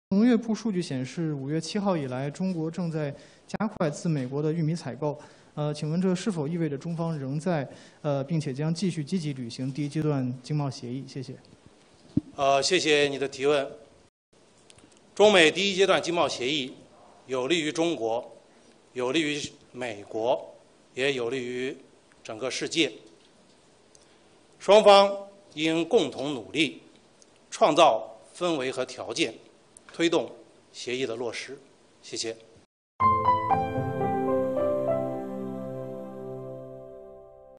商务部回应中美第一阶段经贸协议：双方应共同努力推动协议落实
5月27日，在商务部举行的例行新闻发布会上，有媒体问：美国农业部数据显示，5月7日以来中国正加快自美玉米采购，这是否意味着中方仍在而且将继续积极履行第一阶段经贸协议？对此，商务部新闻发言人高峰表示，中美第一阶段经贸协议有利于中国，有利于美国，有利于整个世界。双方应共同努力，创造氛围和条件，推动协议落实。